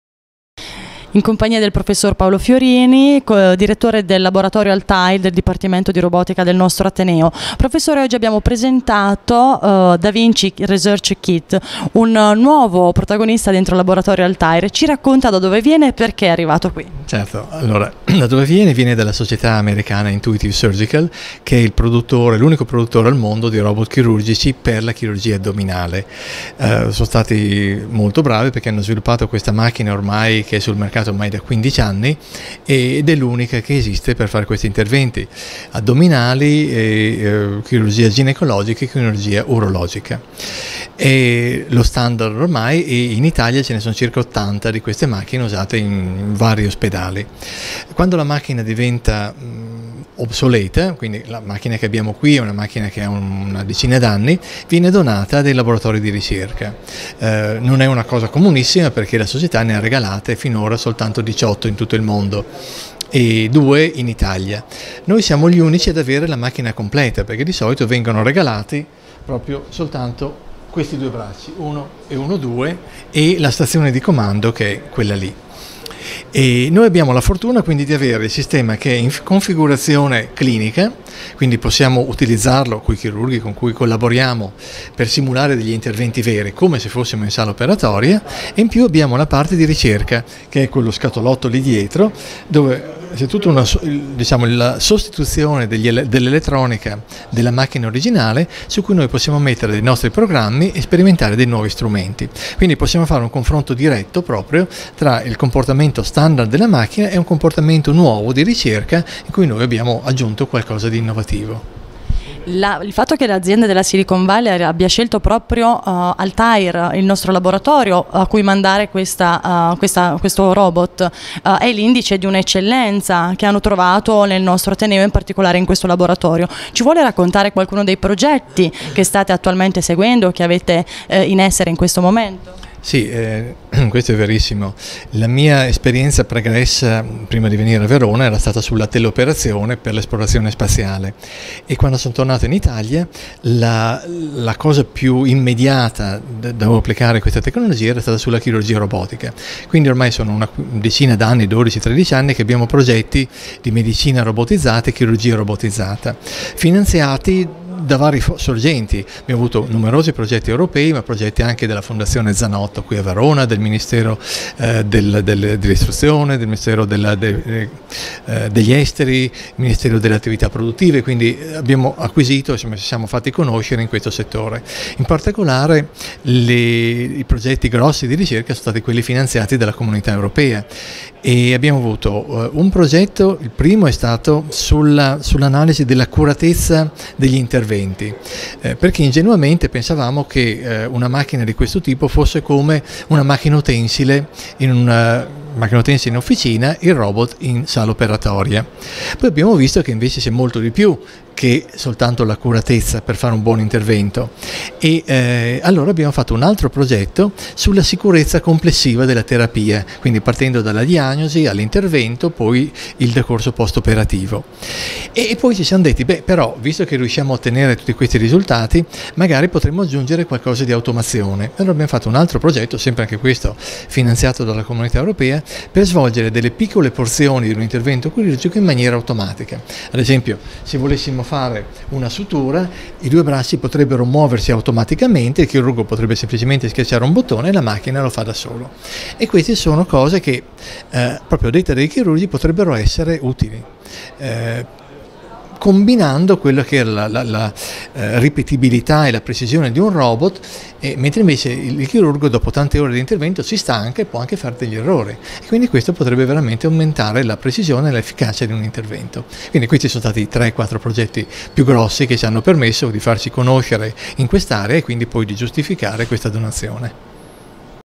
Paolo Fiorini - Altair Robotics Lab
Intervista a Paolo Fiorini, docente del dipartimento di Informatica dell’università di Verona e direttore del Laboratorio di robotica Altair che presenta il “daVinci research kit", robot per la chirurgia addominale minimamente invasiva.